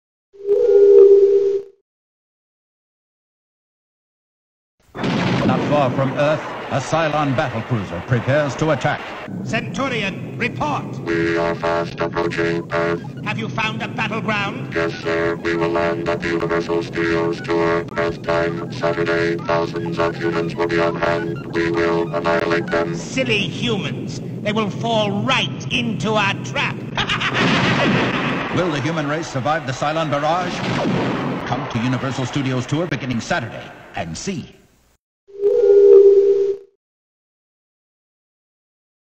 Battle of Galactica - Universal Studios Tour - 1979 Launch Commercial
The launch commercial for the BATTLE OF GALACTICA ride, part of the Universal Studios Tour, noting the date of its premiere to the public.